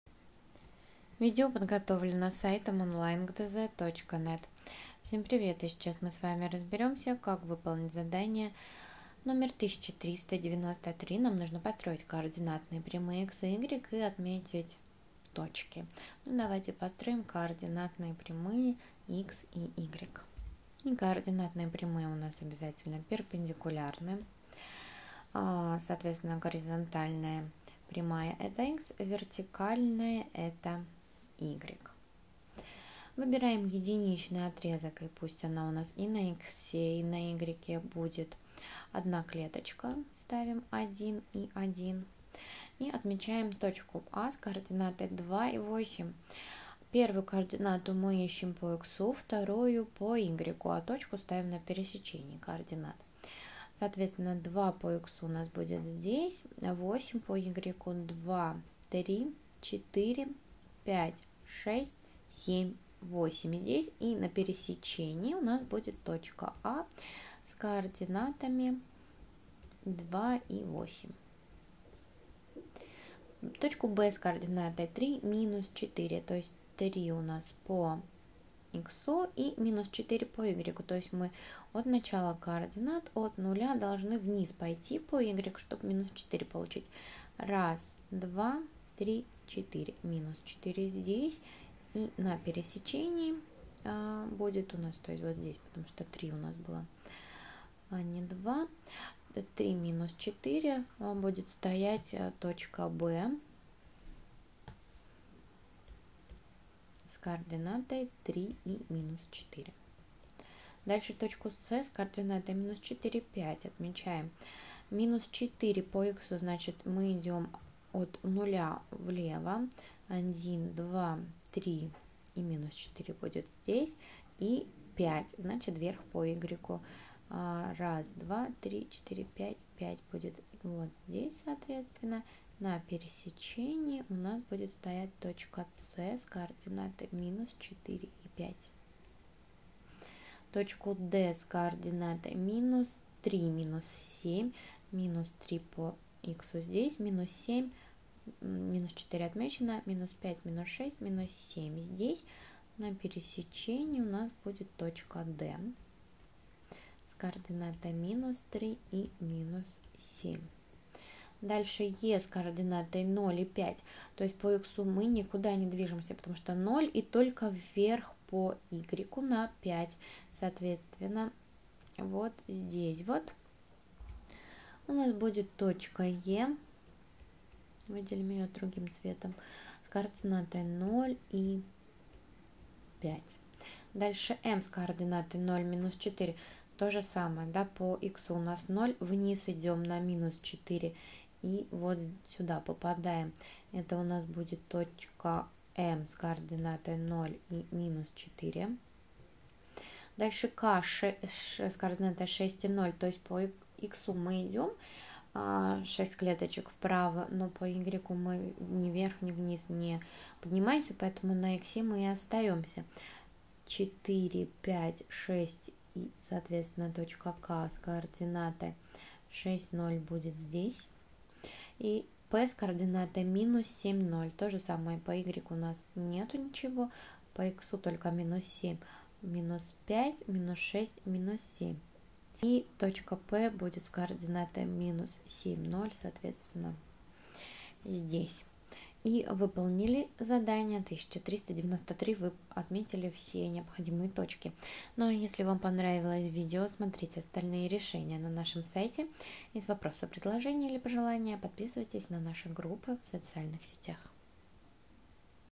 Задание № 1393 - Математика 6 класс (Виленкин, Жохов)
В задании 1393 вам надо построить координатные прямые Х и У и отметить на получившейся координатной плоскости восемь точек с различными координатами. Вашим помощником будет наше видео с решением.